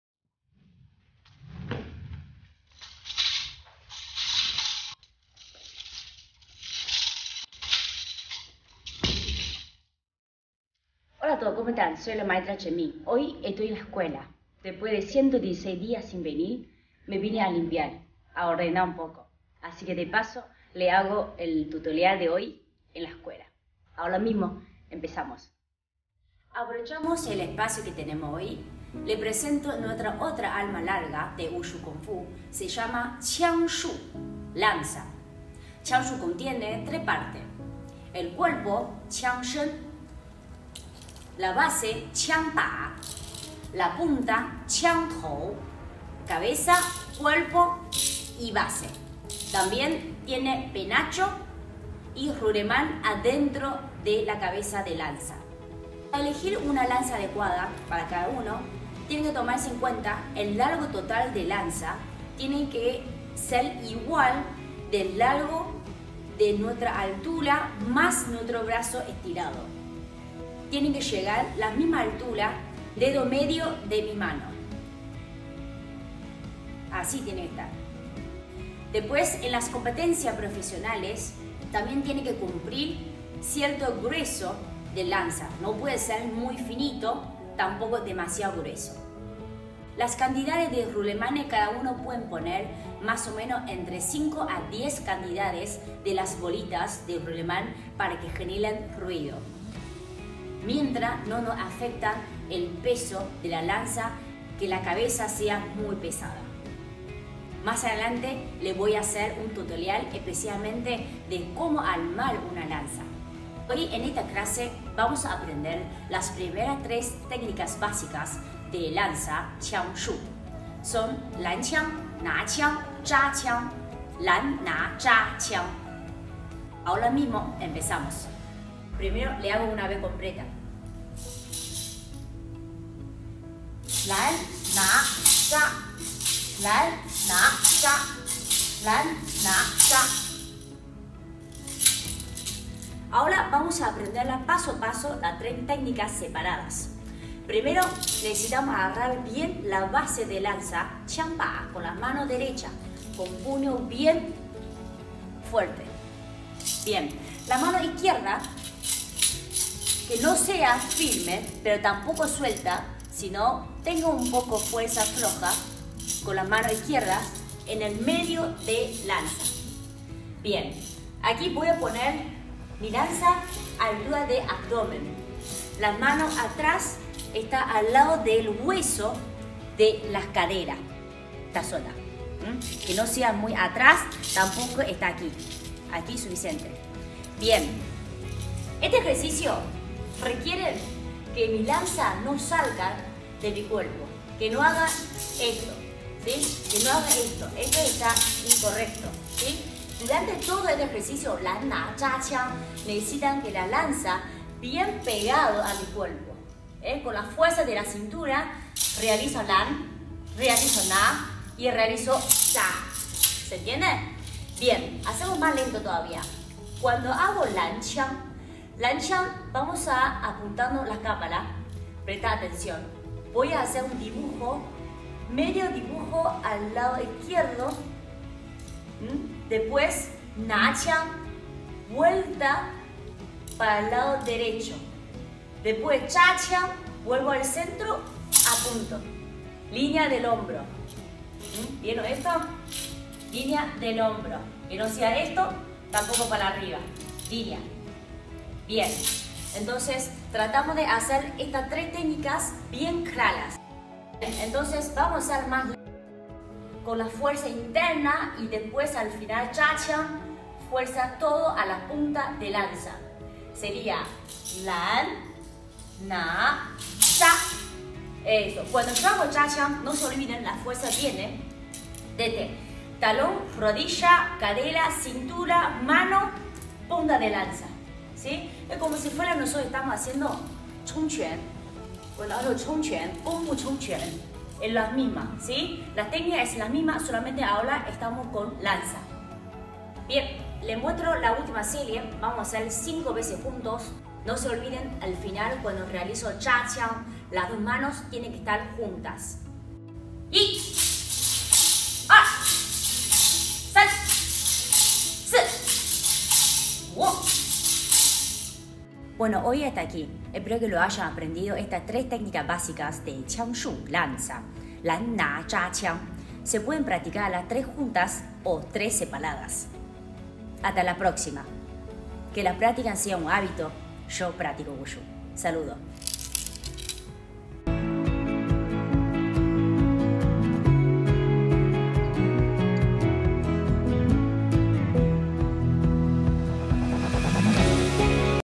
Wushu Kung Fu en Casa Día 116 Lanza 枪术 QiangShu LanNaZha 拦、拿、扎 Spear
*Su donación también será como forma de pago de clase online*
*Your donation will also be as an online class payment method*
Hoy aprovechamos el espacio de la escuela conocemos un poco sobre otra arma larga de Wushu Kung Fu 武术 que requiere fuerza en los antebrazos y la cintura. A practicar!